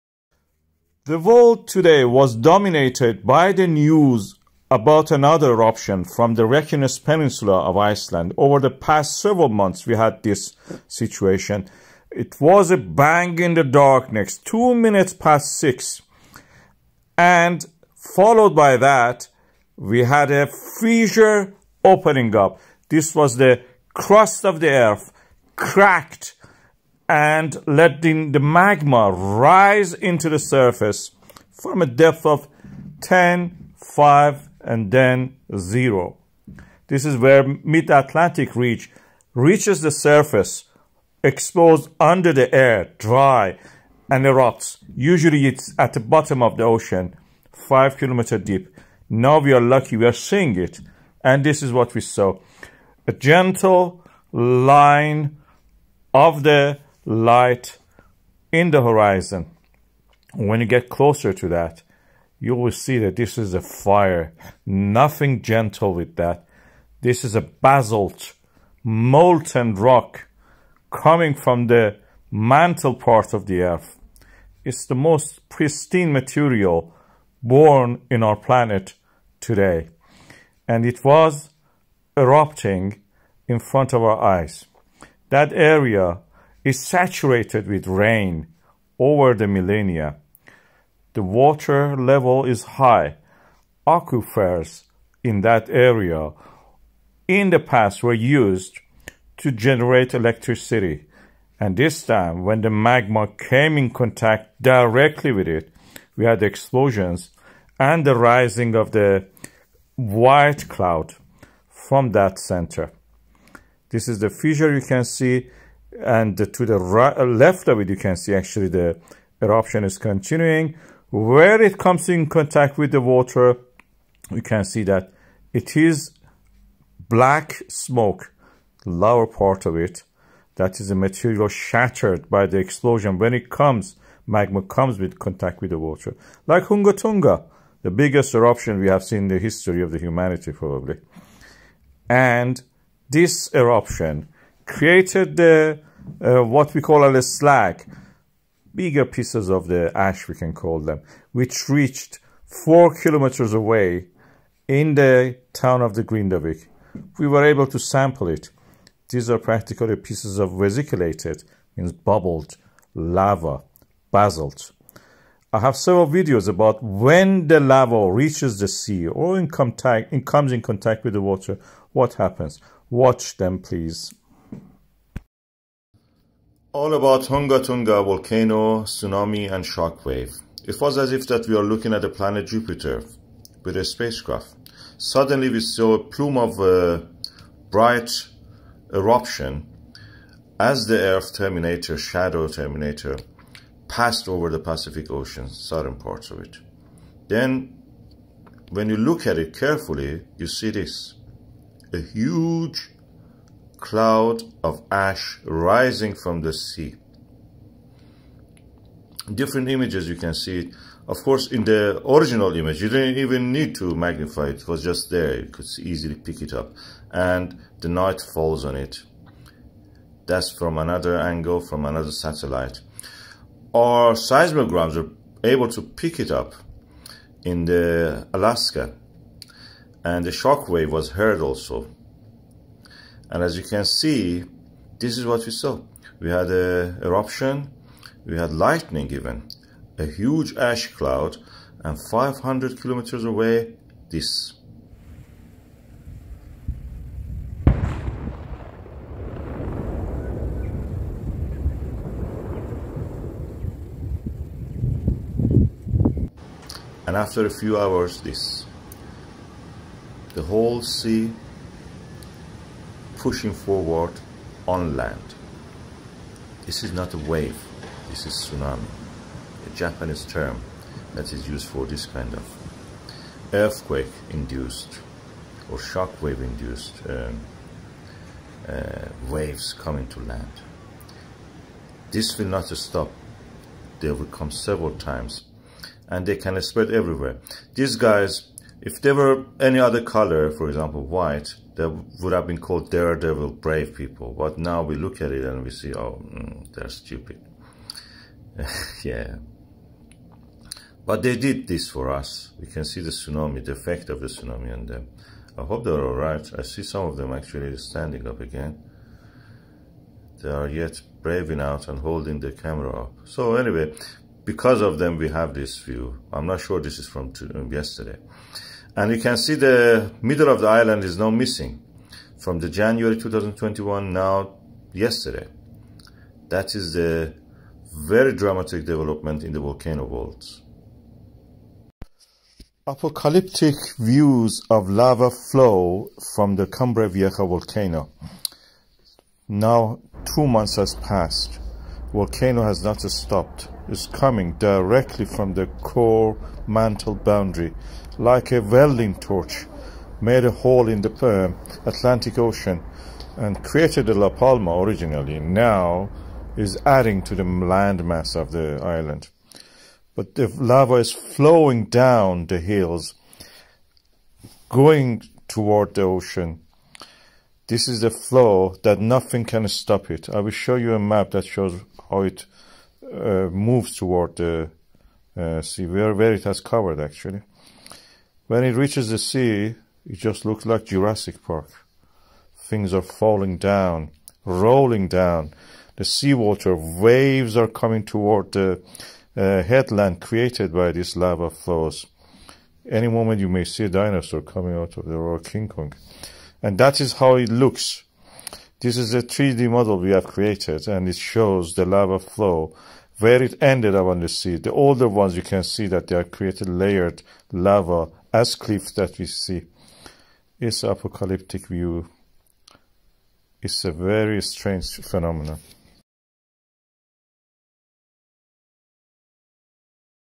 Phreatic Eruption: Lava Exploded Ground Water, Ash Reached Grindavík, Iceland Fissure Volcano
This is an archive material for review. It is not new.